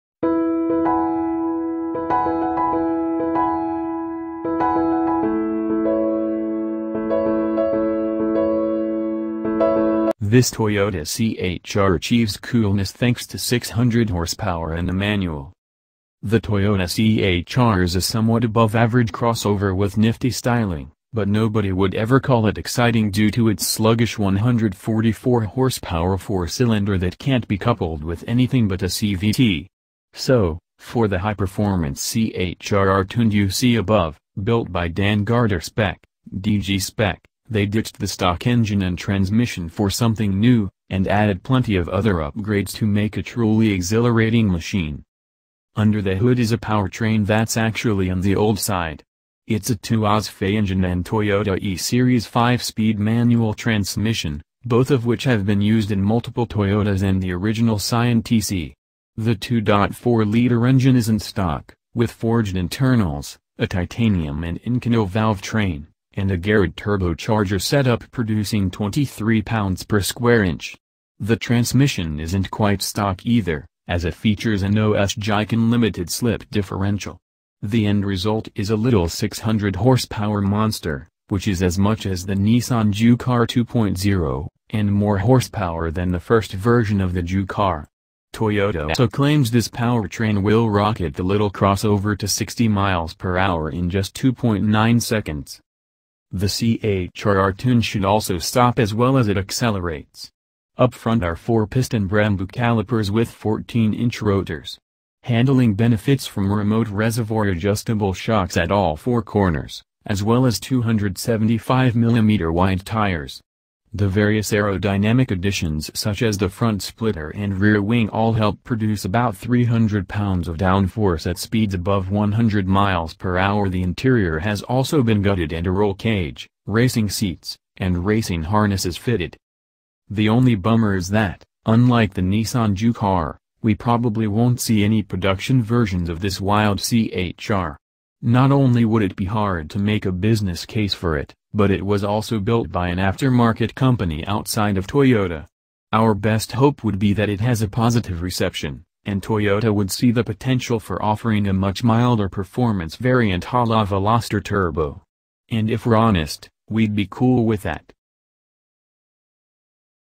2018 Toyota CH R Perfect SUV!!
This Toyota C-HR achieves coolness thanks to 600 hp and a manual

The Toyota C-HR is a somewhat above average crossover with nifty styling, but nobody would ever call it exciting due to its sluggish 144-horsepower four-cylinder that can't be coupled with anything but a CVT. So, for the high-performance C-HR R-Tuned you see above, built by Dan Garder Spec (DG-Spec), they ditched the stock engine and transmission for something new, and added plenty of other upgrades to make a truly exhilarating machine.

Under the hood is a powertrain that's actually on the old side. It's a 2AZ-FE engine and Toyota E-Series 5-speed manual transmission, both of which have been used in multiple Toyotas and the original Scion tC. The 2.4-liter engine isn't stock, with forged internals, a titanium and Inconel valvetrain, and a Garrett turbocharger setup producing 23 psi. The transmission isn't quite stock either, as it features an OS Giken limited-slip differential. The end result is a little 600-horsepower monster, which is as much as the Nissan Juke-R 2.0, and more horsepower than the first version of the Juke-R. Toyota also claims this powertrain will rocket the little crossover to 60 mph in just 2.9 seconds.

The C-HR R-Tuned should also stop as well as it accelerates. Up front are four-piston Brembo calipers with 14-inch rotors. Handling benefits from remote-reservoir adjustable shocks at all four corners, as well as 275-mm wide tires. The various aerodynamic additions such as the front splitter and rear wing all help produce about 300 pounds of downforce at speeds above 100 mph. The interior has also been gutted and a roll cage, racing seats, and racing harnesses fitted.

The only bummer is that, unlike the Nissan Juke-R, we probably won't see any production versions of this wild C-HR. Not only would it be hard to make a business case for it, but it was also built by an aftermarket company outside of Toyota. Our best hope would be that it has a positive reception, and Toyota would see the potential for offering a much milder performance variant a la Veloster Turbo. And if we're honest, we'd be cool with that.